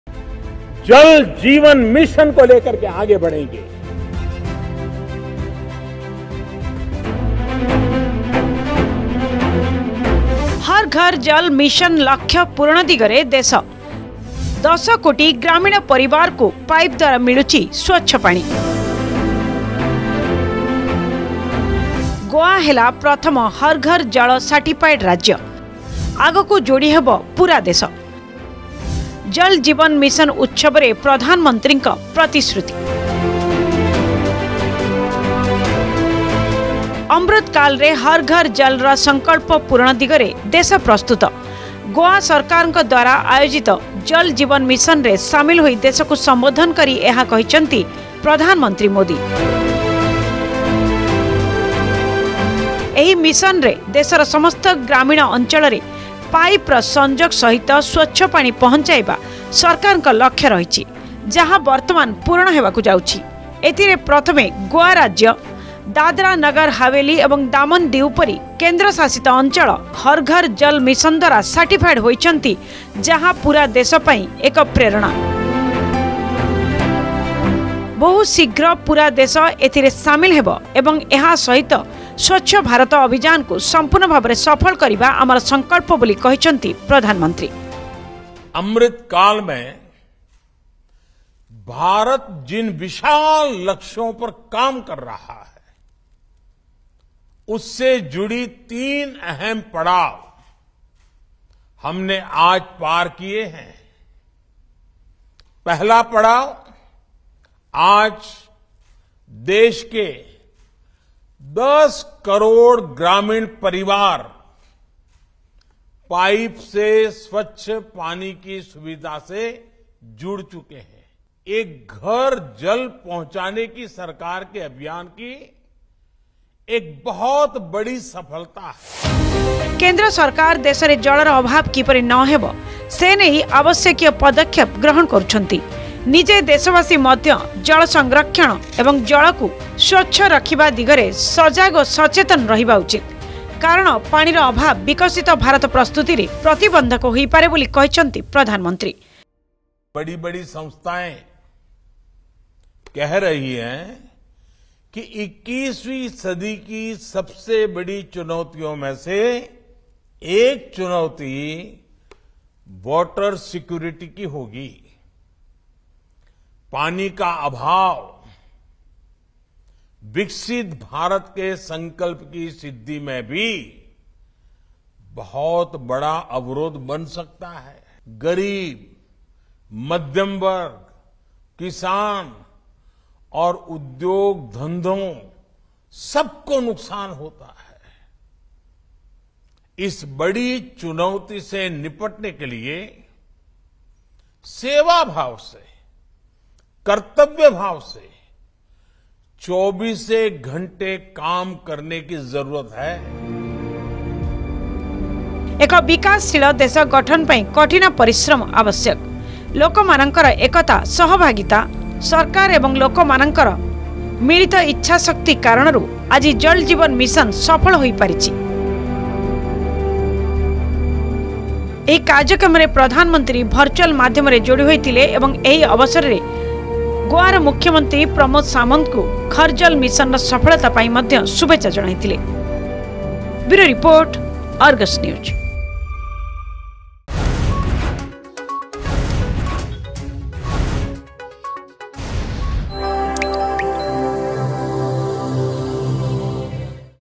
ଗୋଆ ହେଲା ପ୍ରଥମ ହର ଘର ଜଳ ସାର୍ଟିଫାଏଡ ରାଜ୍ୟ
ଦେଶର ଅମୃତକାଳ ରେ ହର ଘର ଜଳ ର ସଂକଳ୍ପ ପୂରଣ ଦିଗରେ ଦେଶ ପ୍ରସ୍ତୁତ ା ଗୋଆ ସରକାରଙ୍କ ଦ୍ବାରା ଆୟୋଜିତ ଜଳ ଜୀବନ ମିଶନରେ ସାମିଲ ହୋଇ ଦେଶକୁ ସମ୍ବୋଧନ କରି ଏହା କହିଛନ୍ତି ପ୍ରଧାନମମନ୍ତ୍ରୀ ମୋଦି